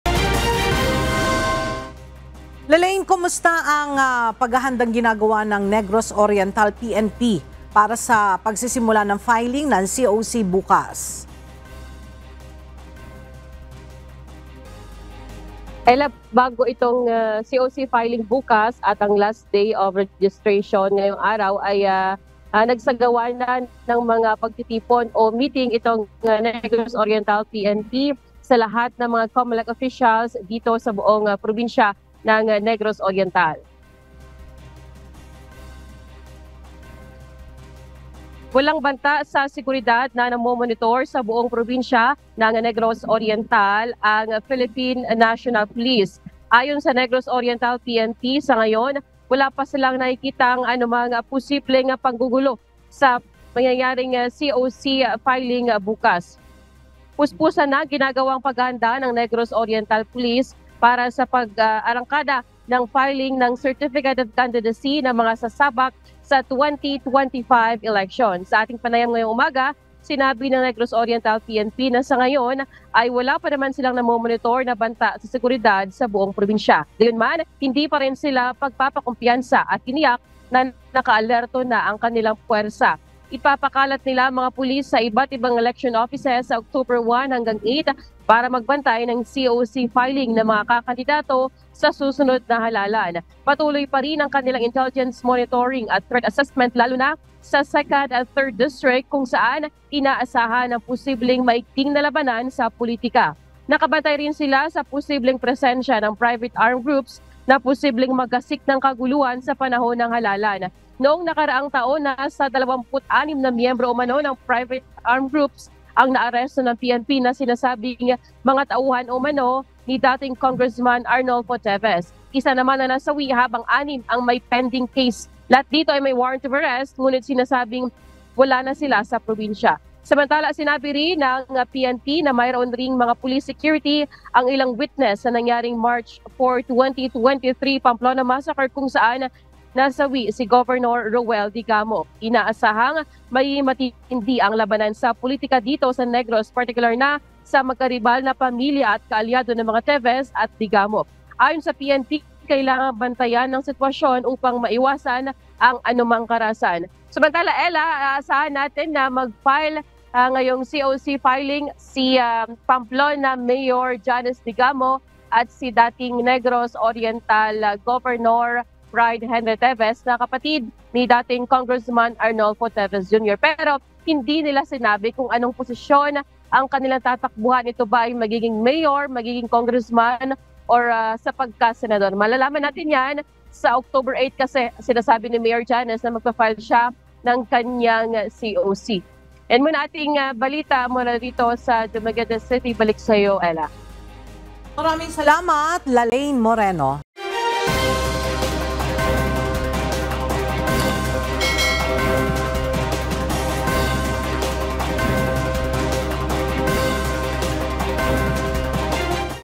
Negros Oriental PNP, nakaalerto na para sa COC filing; posibleng private armies, binabantayan
Mahigpit na binabantayan ngayon ng puwersa ng Negros Oriental Police ang mga aktibidad na may kinalaman sa nalalapit na 2025 elections, partikular na ang pagsisimula ng filing ng Certificates of Candidacy bukas, Oct. 1.

Nagsasagawa na rin sila ng intelligence monitoring sa posibleng presensya ng private armies at banta sa seguridad.